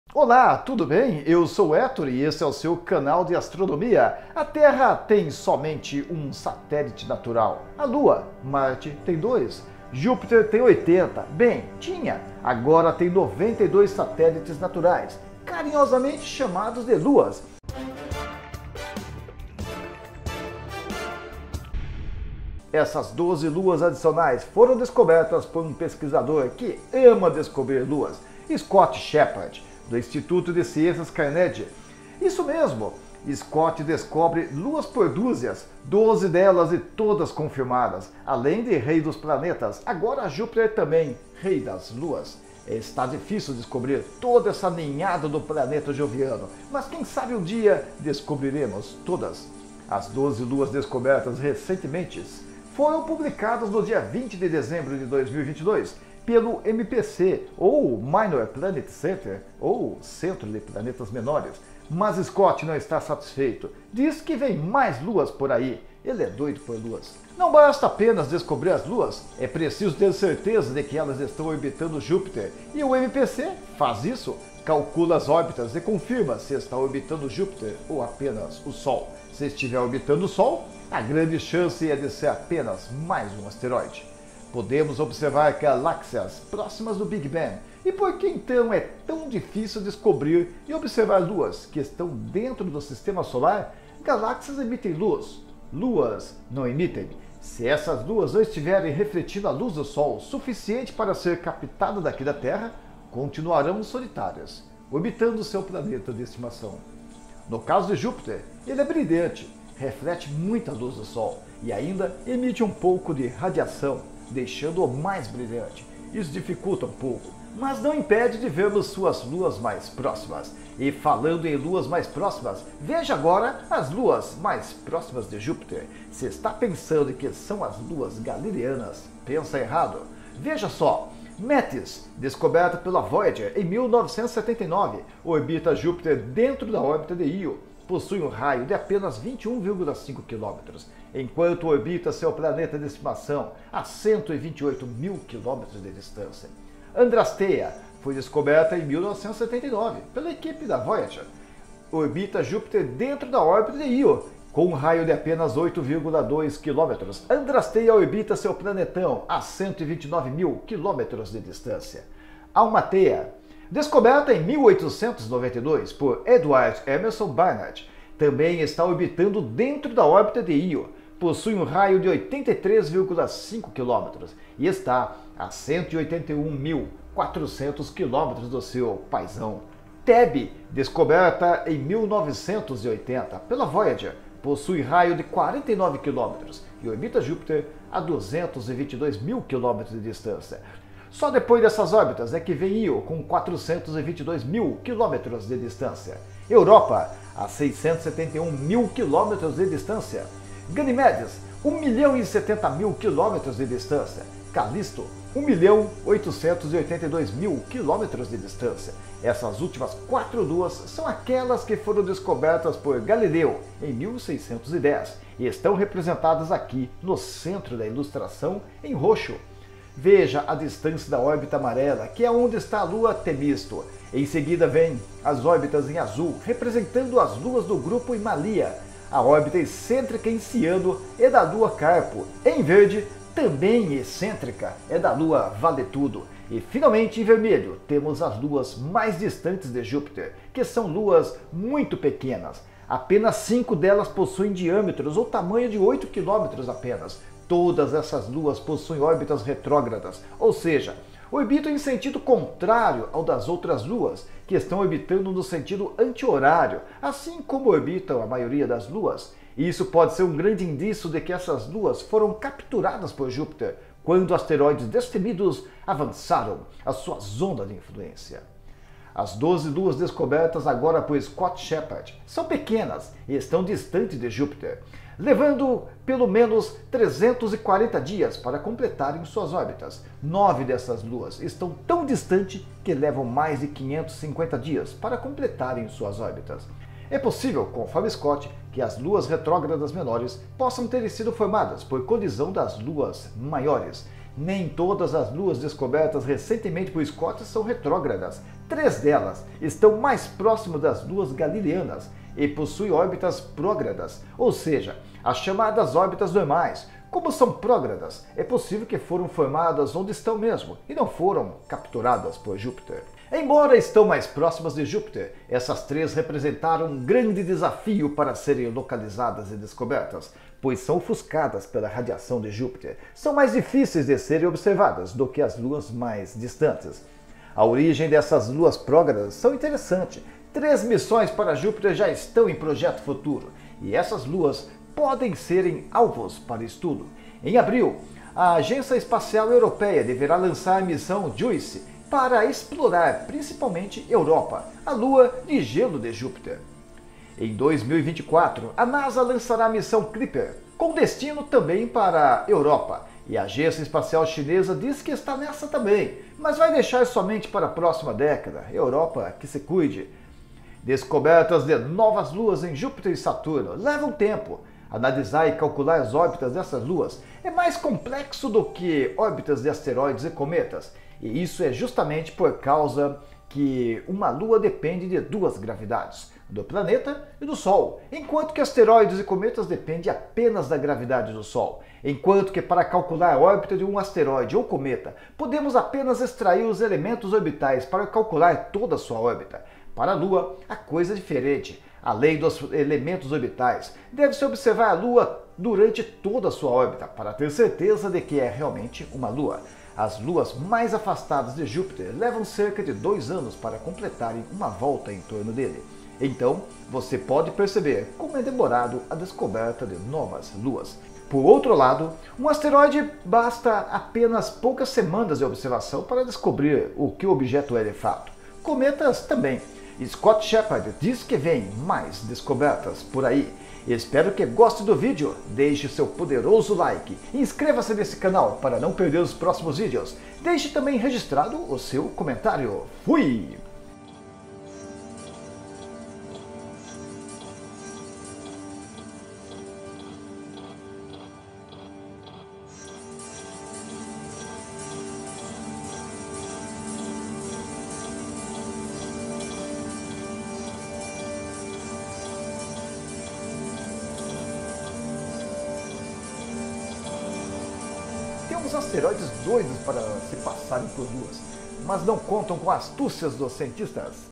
Júpiter tem agora 92 luas e pode ter mais!
Júpiter tinha 80 luas, agora tem 92!
Observação: É perfeitamente correto chamar satélites naturais de luas (com "l" minúsculo) em referência à nossa Lua (com "L" maiúsculo).

Como apoiar o canal:
Seja membro deste canal para apoiá-lo:

                        /    .  Y                            .
                       /      -_j                      +
        .             Y     ( --l__
                      l        _)
     .                 \     '"l,